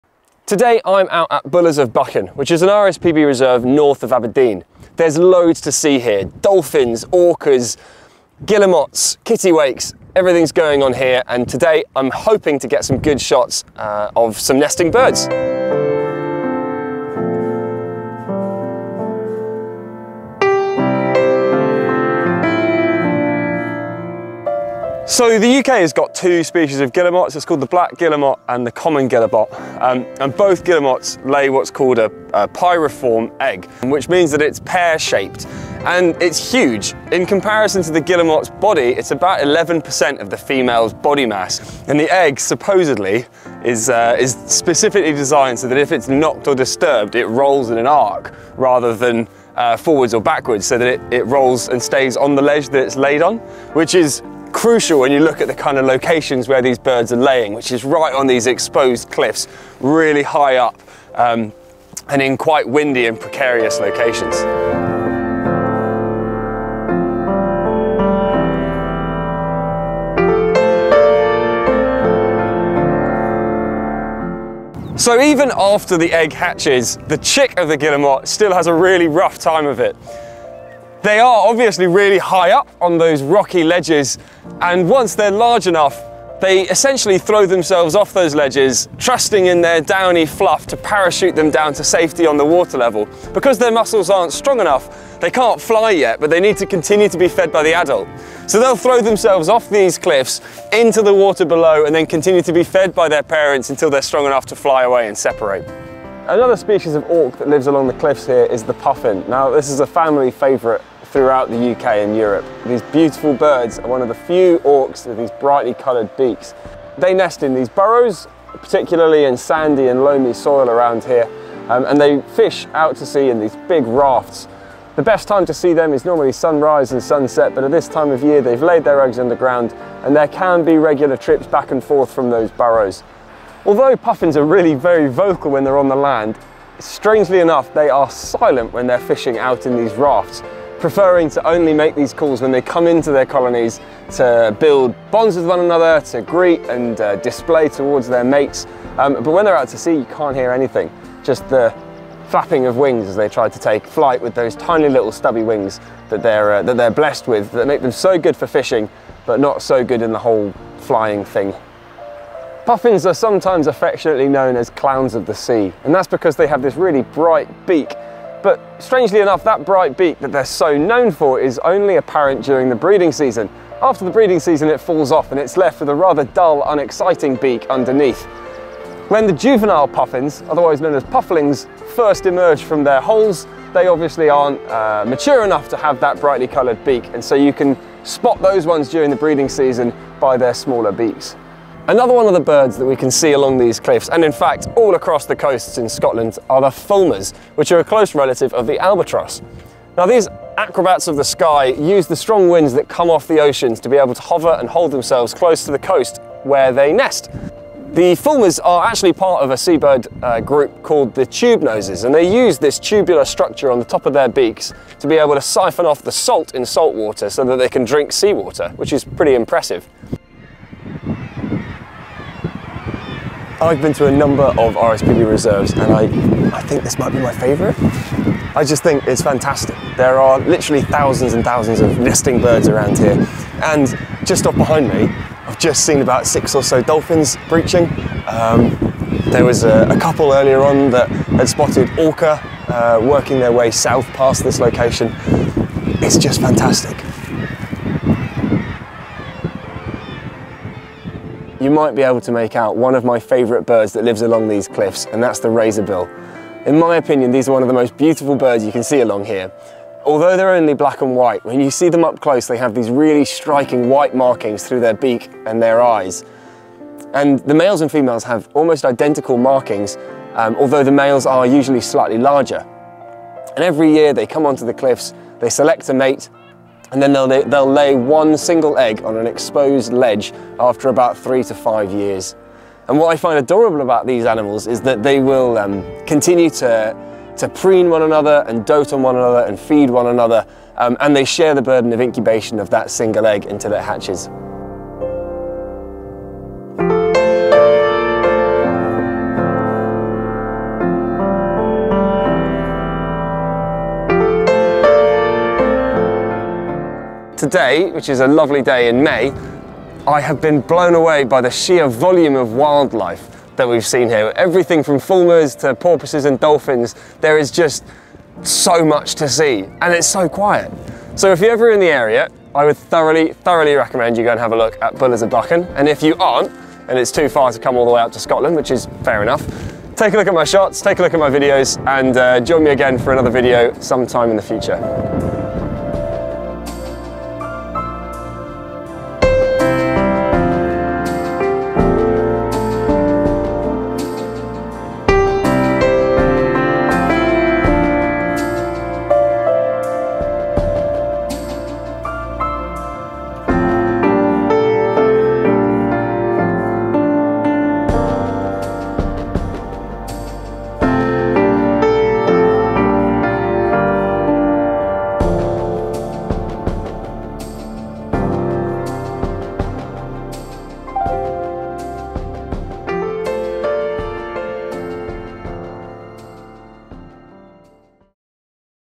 Visiting Scotland's hidden wildlife location - Bullers of Buchan
Bullers of Buchan is an amazing RSPB reserve north of Aberdeen. From these rocky outcrops you can see thousands of nesting seabirds, cetaceans and native flora. So get your binoculars ready and head to the cliffs. I hope you enjoy the show!